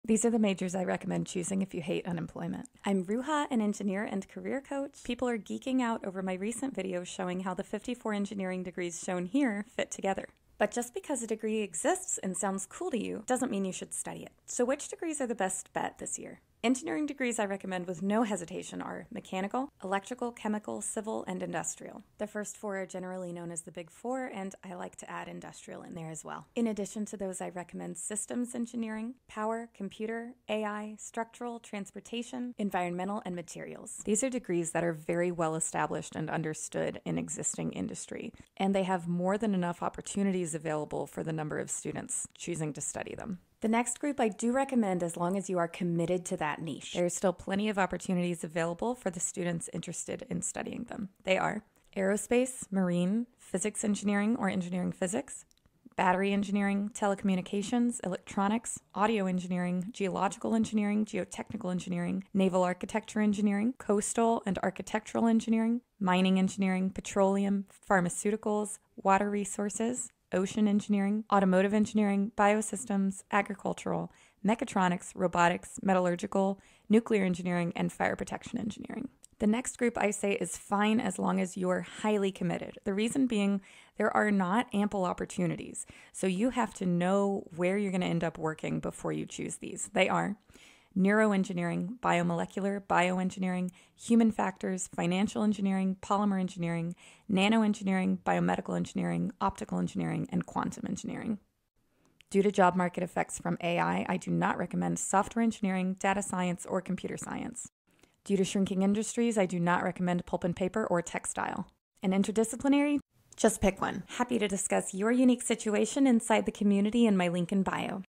Which engineering college degrees are the best to study in 2025? Let me know in the comments
The hiring landscape for 2025 is different than any time in recent history. Looking ahead, how do you think the engineering fields measure up?